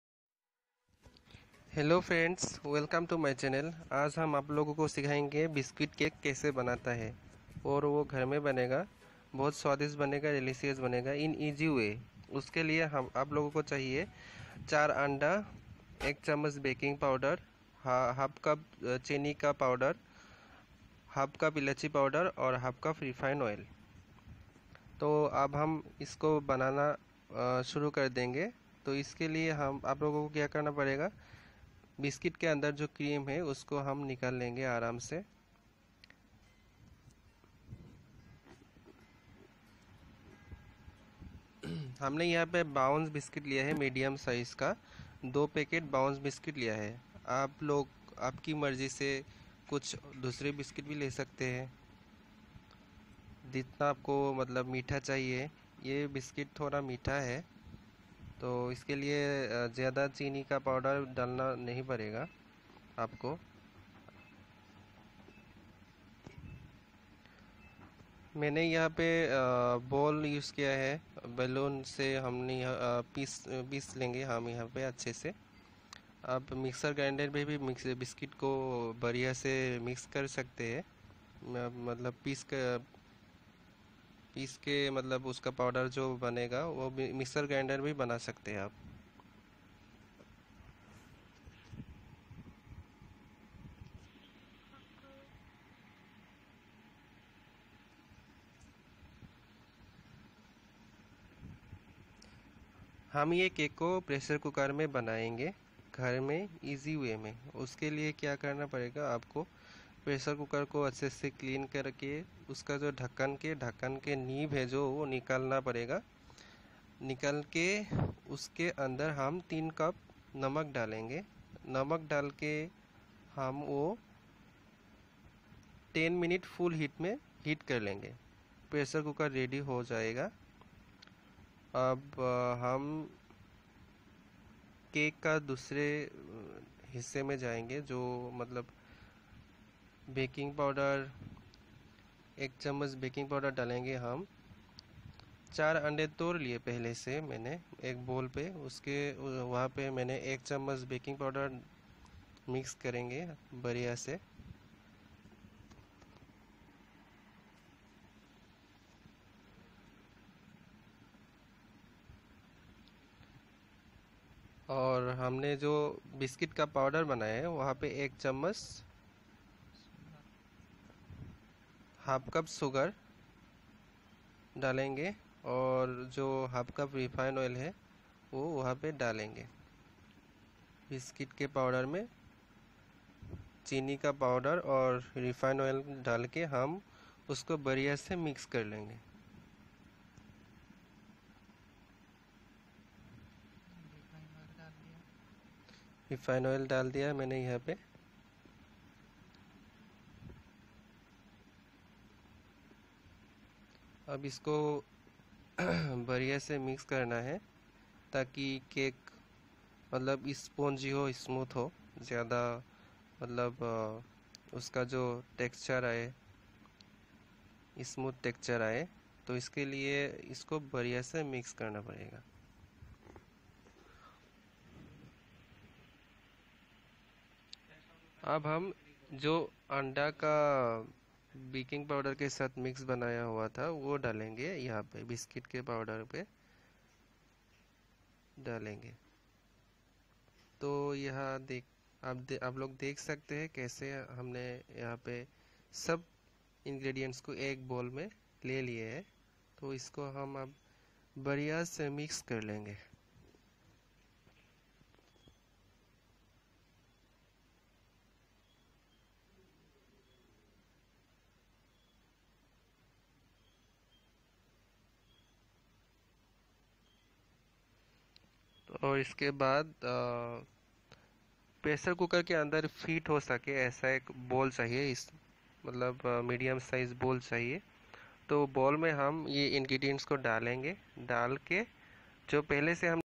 Biscuit cake only 5 ingredients in lock -down without egg , oven, maida
Biscuit cake only 5 ingredients in lock -down without egg, oven maida , Home biscuit recipe, cake , egg ,without oven
How to make biscuit ckake at home###
Cook- nazima yeasmin
Voice - Moshiur Islam
Editors - Moinul Islam
### How to make a better cake $##

[ INGREDIENTS ]
1. TWO PACK BOUNCE or others sweet BISCUIT
2. FOUR EGG
3. 1/2 SPOON ELACHI POWDER
4. ONE SPOON BACKING POWDERS
5. 1/2 CUP SUGGAR

#GOOD LUCK###